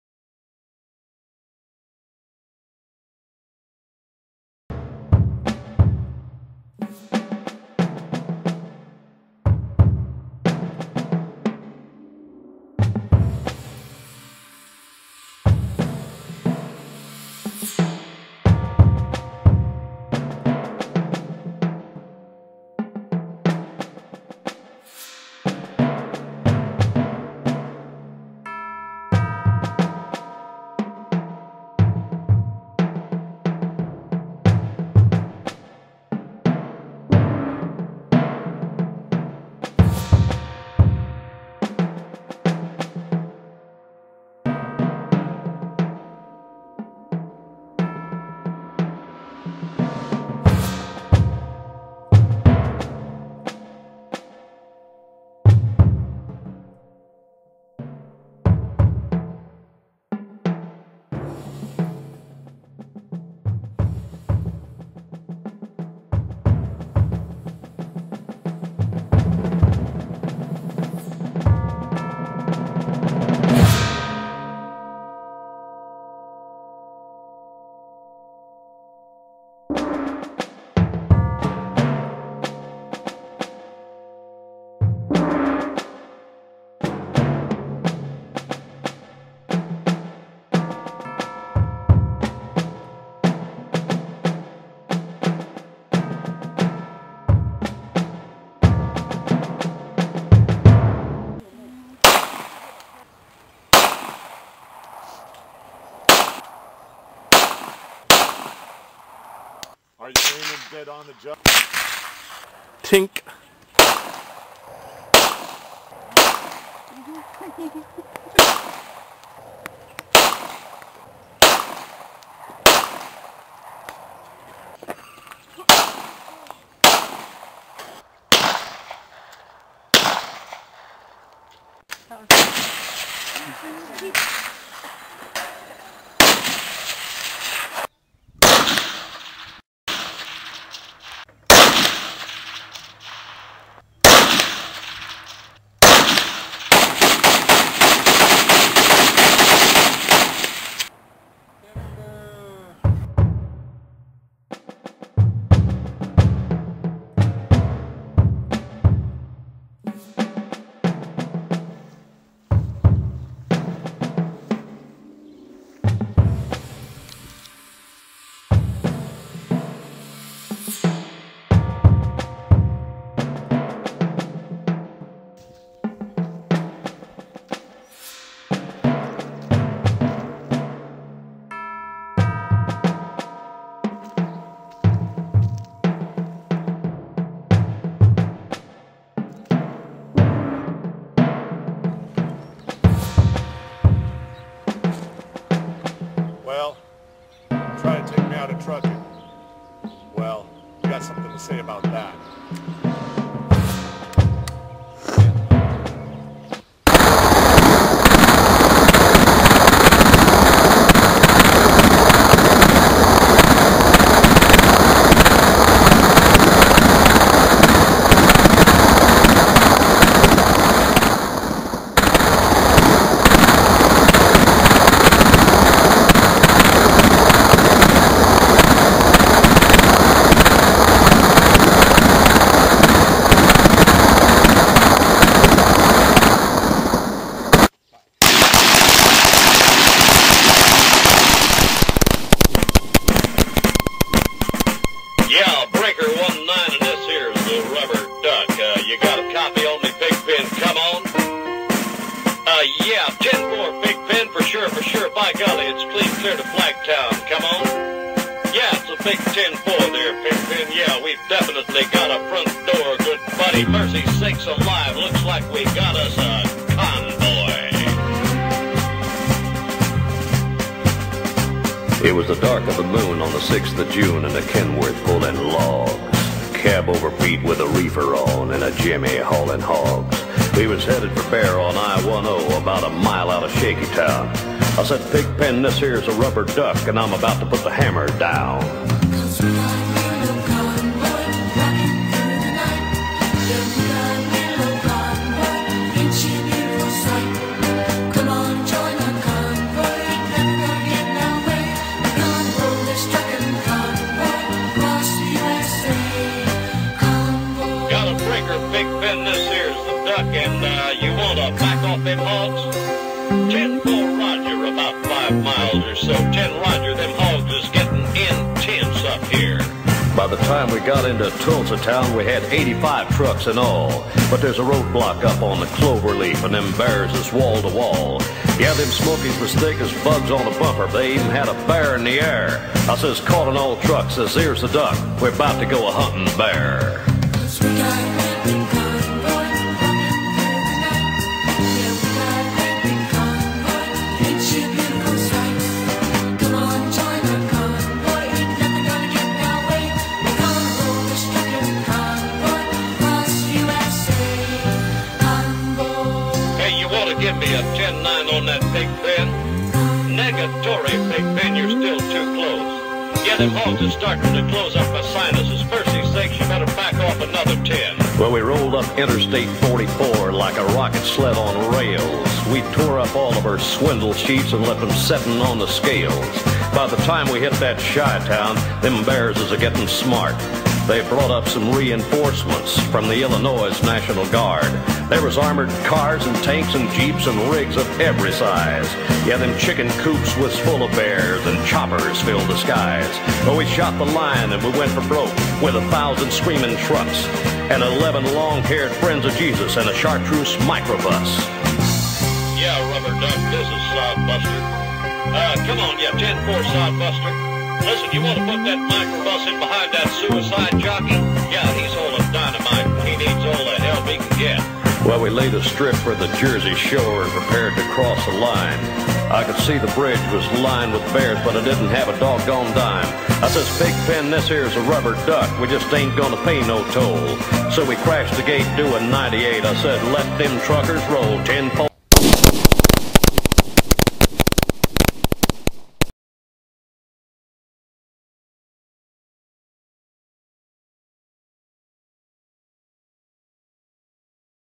CONVOY 8 truckers law
The Convoy gets in gun fight with DOT and officer Holding Johnson is stuck playing with his balls. Roll on Truckers Roll on.